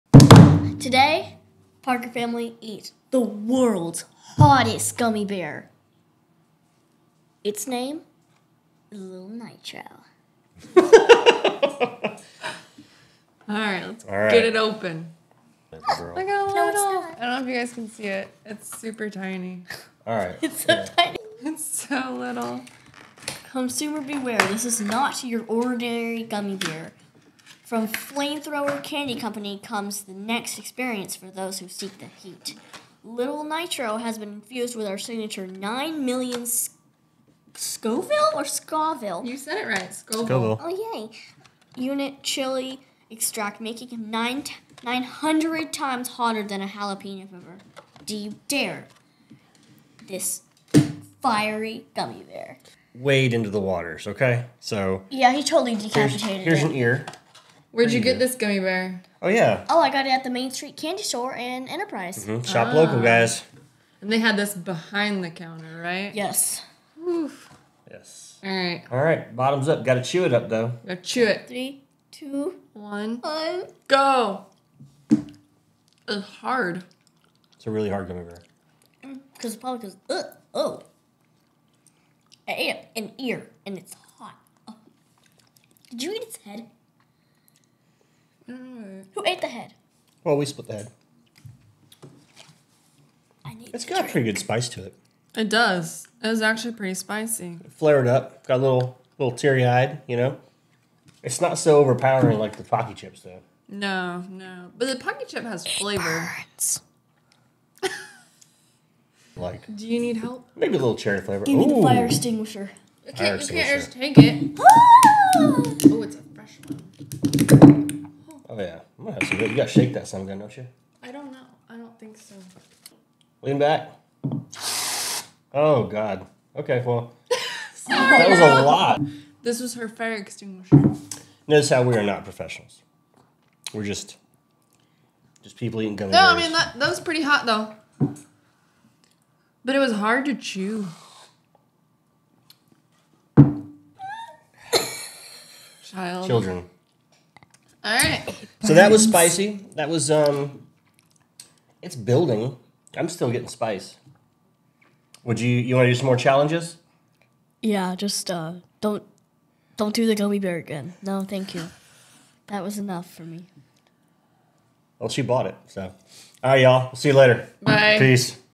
Parker Family Tries Lil' Nitro World's Hottest Gummy Bear!
Today The Parker Family Tries...
Lil' Nitro - The World's Hottest Gummy Bear!

Will this one make Britney cry? Can Austin maintain some semblance of a show flow? Why did TheGirl need a fireextinguisher??!!